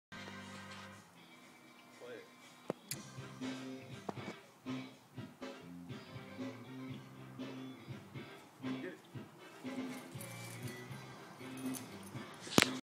German Shorthaired Pointer jumps over 7ft.
Costa, the 3 years old German Shorthaired Pointer jumps for his toy.